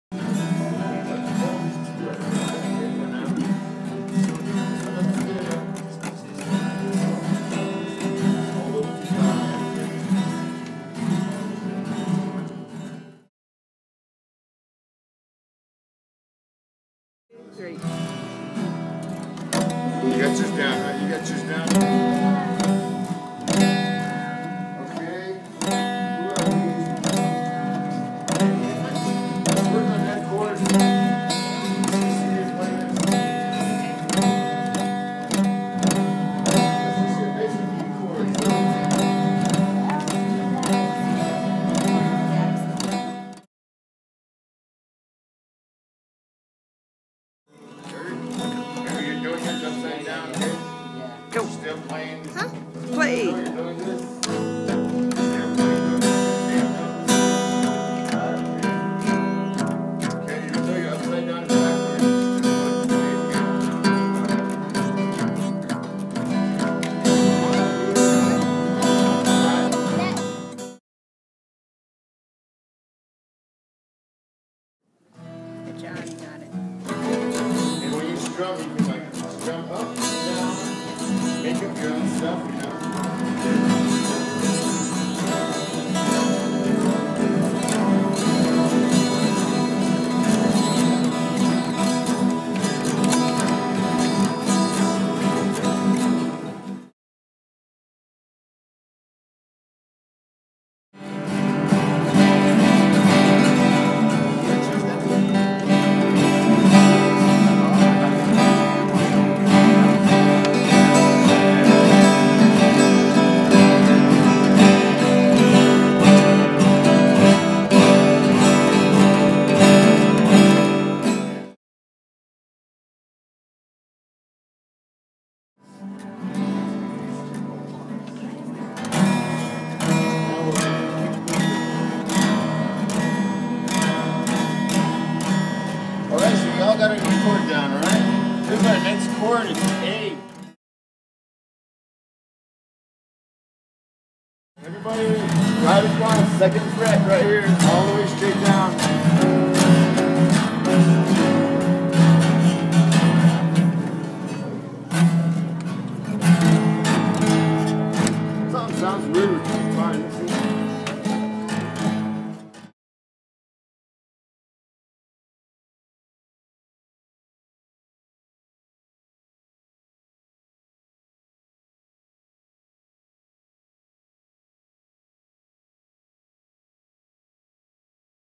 "Teens For Music" March 7, 2015
Jeff J Lee from Muddy Soul'z volunteers to help the children learn guitar. These children love music.